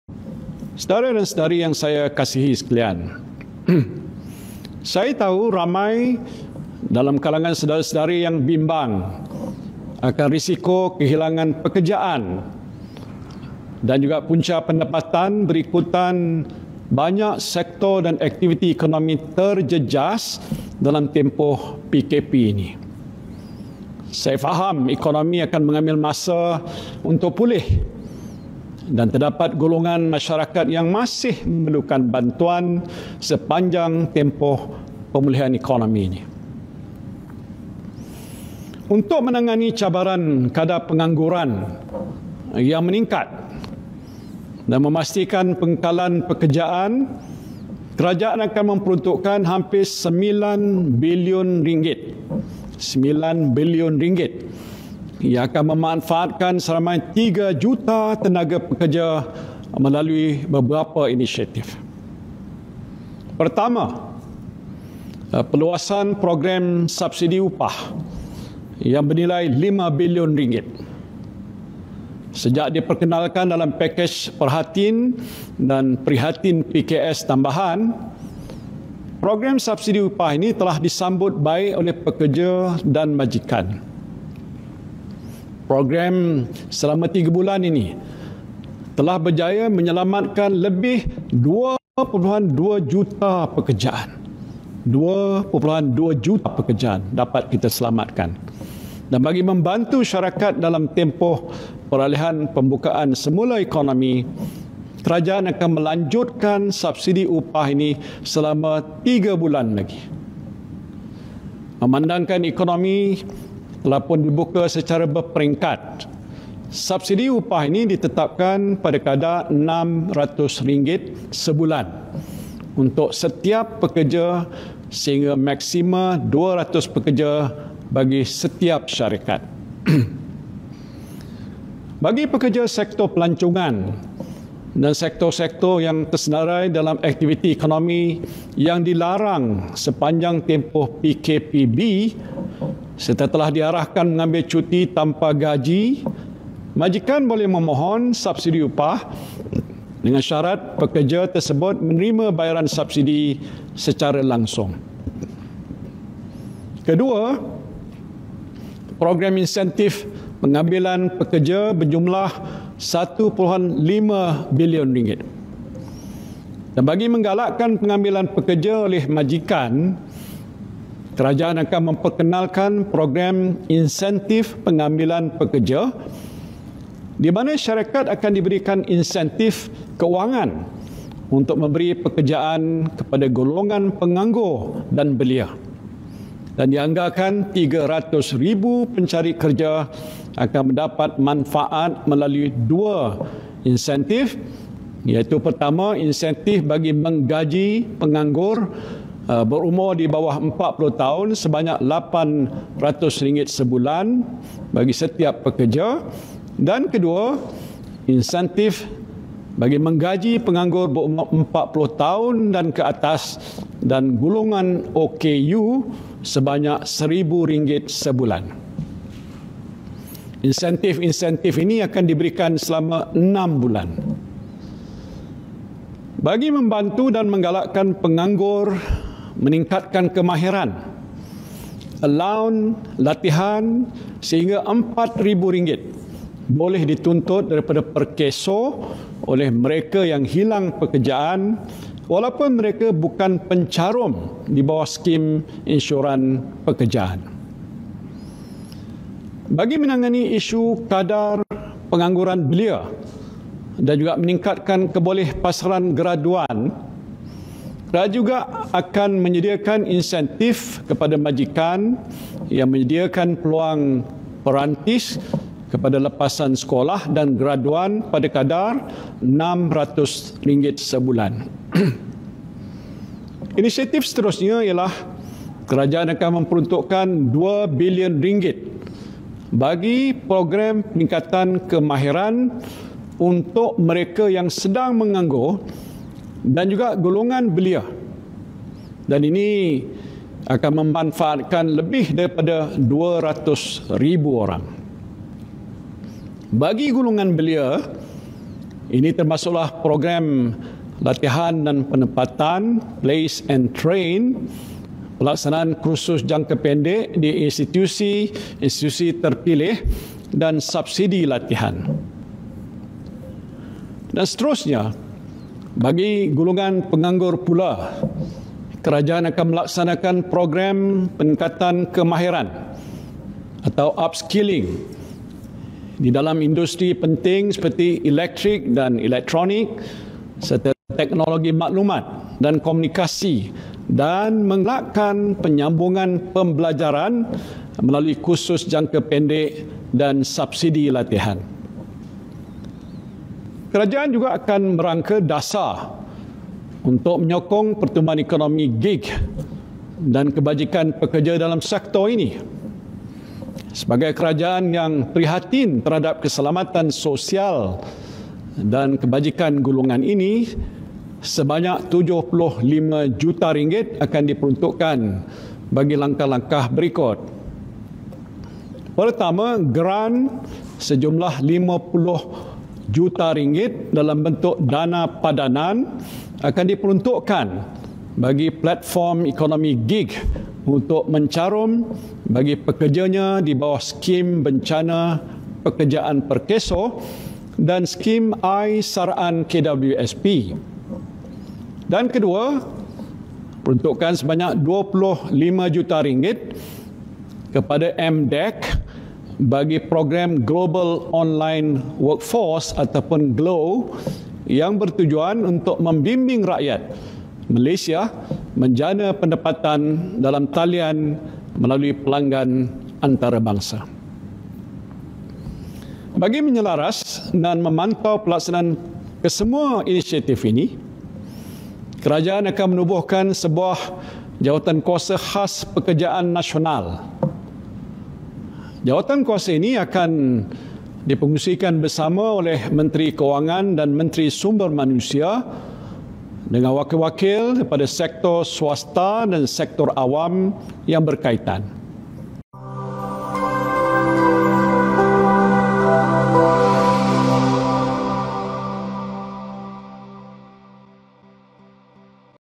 RM9 Billion kerajaan peruntukkan untuk pastikan rakyat tidak menganggur
Kerajaan Malaysia prihatin rakyat. RM9 billion diperuntukkan untuk menghadapi cabaran pengangguran dan mengekalkan pekerjaan rakyat.